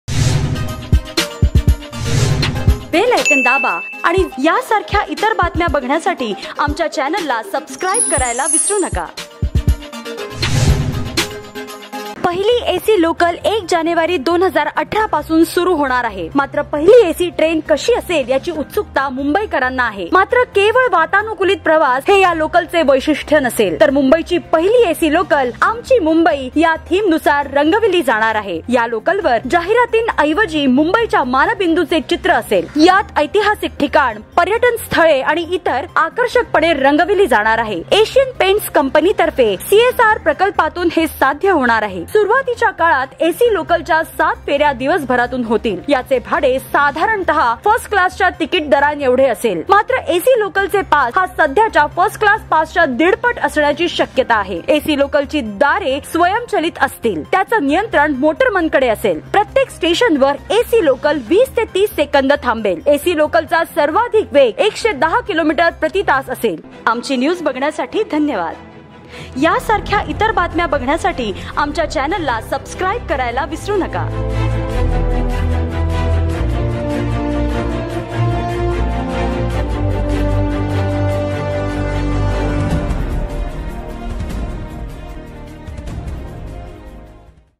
ही' असणार आहे मुंबईकरणांसाठी नवीन वर्षाची नवी भेट | Mumbai Latest News | Lokmat Marathi News
पहिली एसी लोकल 1 जानेवारी 2018 पासून सुरू होणार आहे. मात्र पहिली एसी ट्रेन कशी असेल याची उत्सुकता मुंबईकरांना आहे. मात्र केवळ वातानुकुलीत प्रवास हे या लोकलचे वैशिष्टय़ नसेल. तर मुंबईची पहिली एसी लोकल ‘आमची मुंबई’ या थीमनुसार रंगवली जाणार आहे. या लोकलवर जाहिरातींऐवजी मुंबईच्या मानबिंदूंचे चित्र असेल. यात ऐतिहासिक ठिकाण, पर्यटन स्थळे आणि इतर आकर्षकपणे रंगवली जाणार आहेत. एशियन पेंट्स कंपनीतर्फे सीएसआर प्रकल्पातून हे साध्य होणार आहे. एशियन पेंट्स आणि स्ट्रीट आर्ट इंडिया यांच्या समन्वयाने हा प्रकल्प मार्गी लागेल. सुरुवातीच्या काळात एसी लोकलच्या सात फे-या दिवसभरातून होतील. याचे भाडे साधारणत: फस्ट क्लासच्या तिकीट दरांएवढे असेल. मात्र एसी लोकलचे पास हा सध्याच्या फर्स्ट क्लास पासच्या दीडपट असण्याची शक्यता आहे. एसी लोकलची दारे स्वयंचलित असतील. त्याचे नियंत्रण मोटरमनकडे असेल. प्रत्येक स्टेशनवर एसी लोकल 20 ते 30 सेकंद थांबेल. एसी लोकलचा सर्वाधिक वेग 110 किमी प्रती तास असेल. 2012-13 च्या बजेटमध्ये पहिल्यांदा मुंबईत एसी लोकल धावण्याचे नमूद केले गेले.